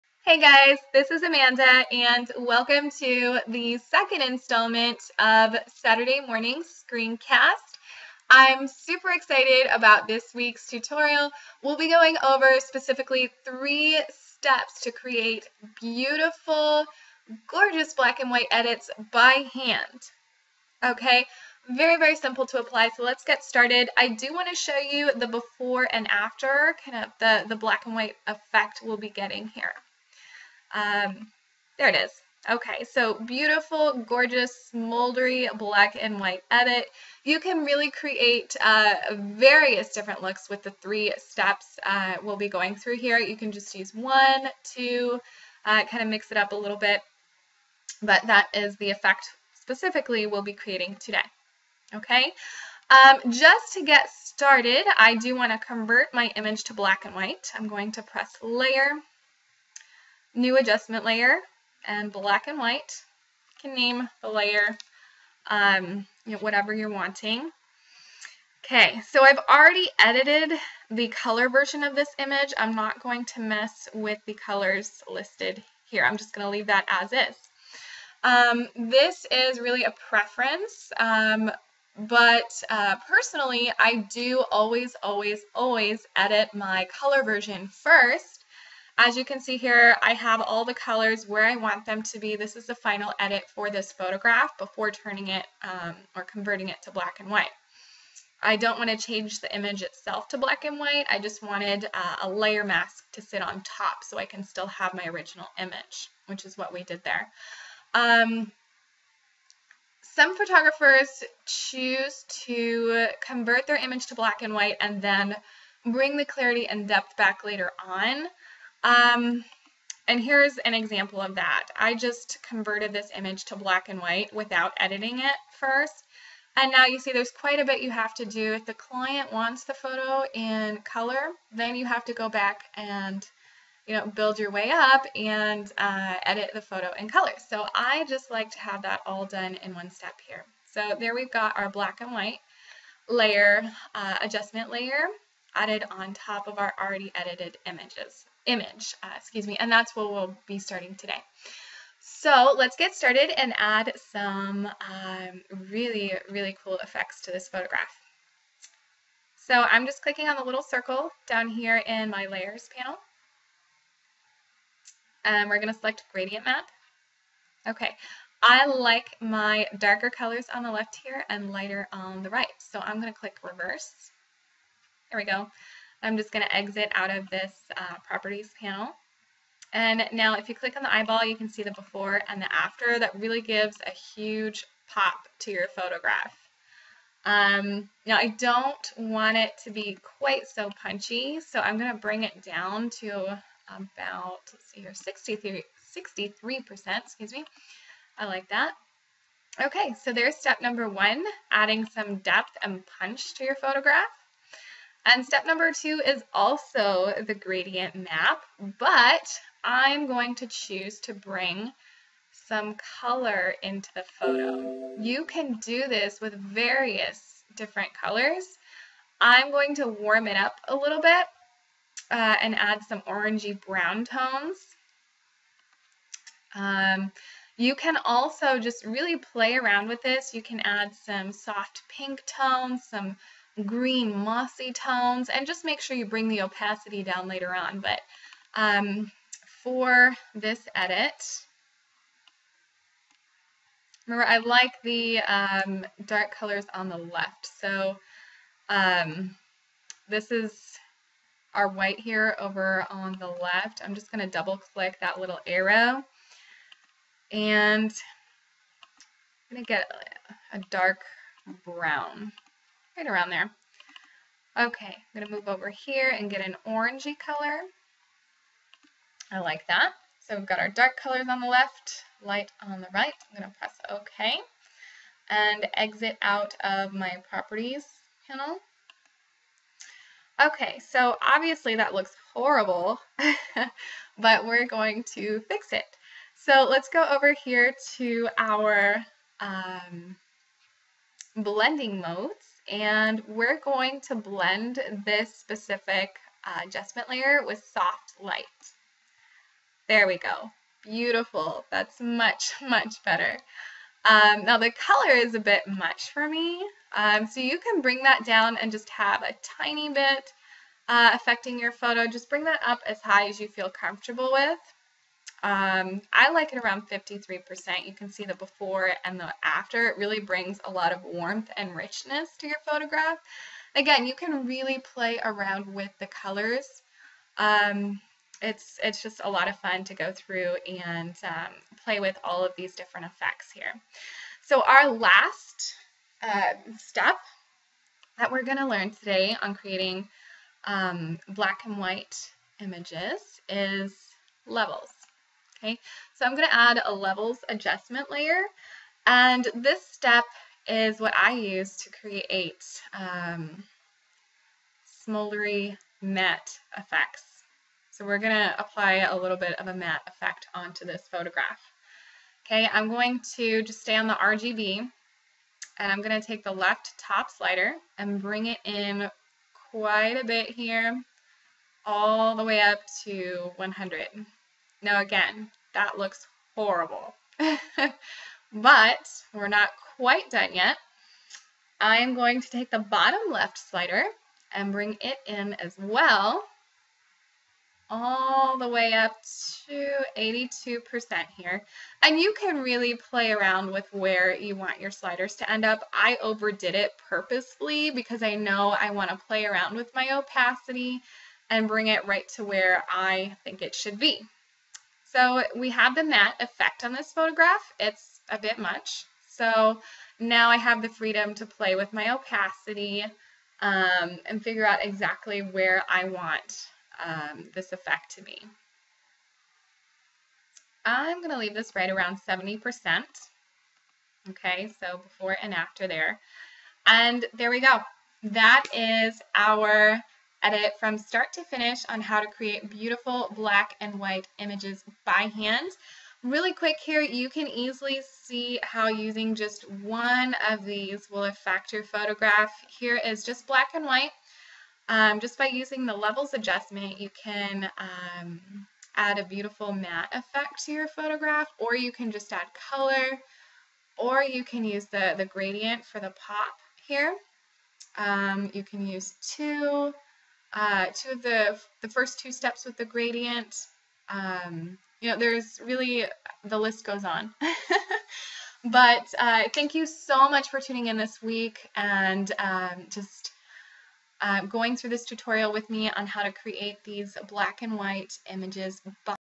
3 Simple Steps to Create Beautiful Black and White Images in Photoshop | A Photoshop Tutorial
3 simple steps to creating gorgeous black and white images by hand in Photoshop! These steps are applicable for any version of Photoshop. This is a must watch and quickly becoming a fan favorite!